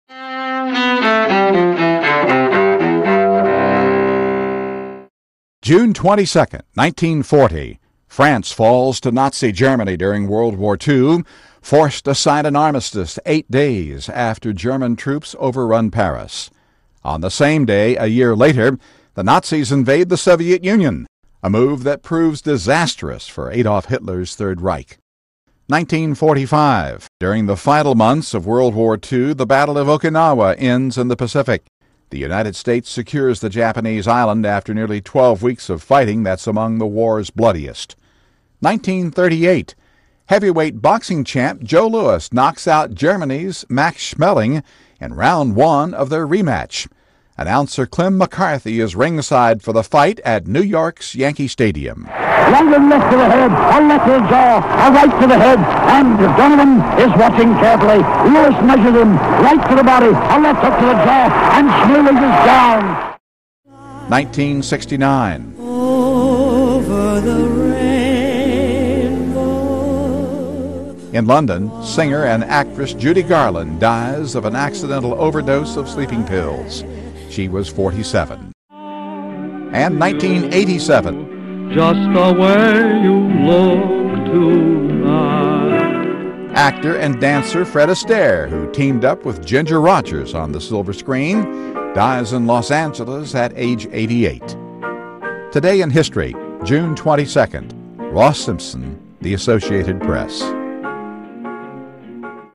Today in History for June 21
Highlights of this day in history:  Three civil rights workers disappear in Mississippi; John Hinckley, Jr. found not guilty by reason of insanity for shooting President Ronald Reagan and three others; Britain's Prince William born.  (June 21)